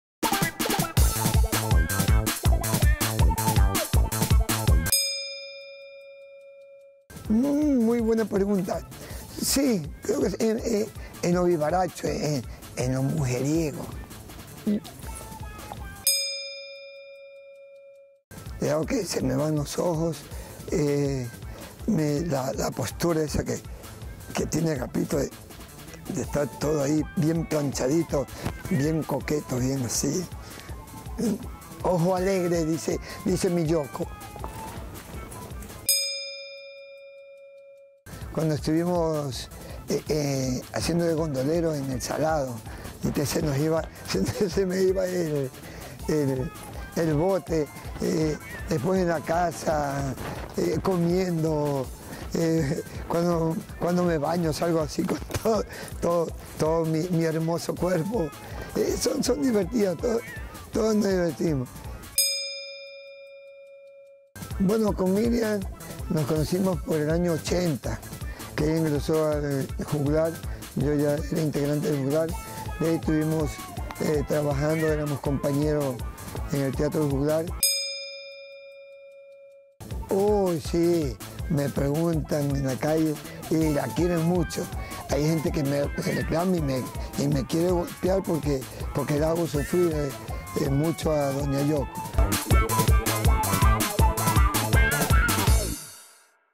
3 Familias | Entrevista Isidro Murillo - ¿El personaje Agapito se parece a Isidro? | 4ta temporada
Todo lo que necesitas saber sobre 3Familia, 4ta temporada lo encontrarás

3 Familias | Entrevista Isidro Murillo - ¿El personaje Agapito se parece a Isidro? | 4ta temporada